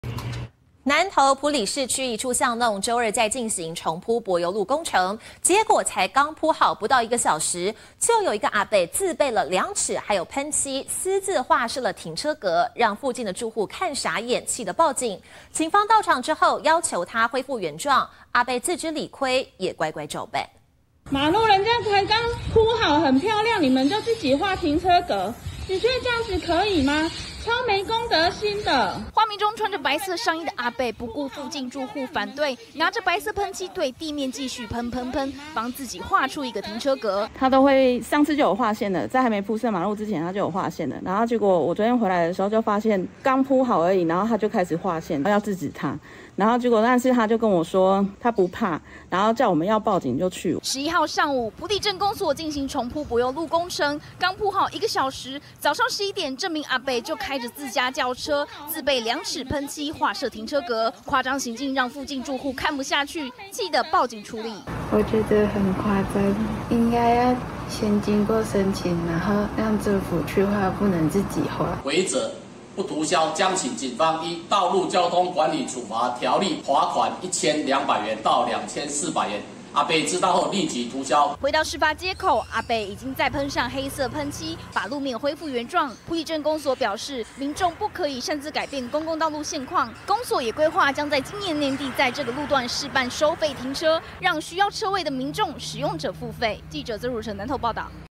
私劃停車格可罰2400元 阿伯塗銷恢復原狀
南投埔里市區一處巷弄，週二進行重鋪柏油路工程，沒想到剛餔好不到1小時，就有阿伯自備量尺跟噴漆，私自畫設停車格，讓附近住戶看傻眼，警方到場，要求恢復原狀，阿伯自知理虧，乖乖照辦。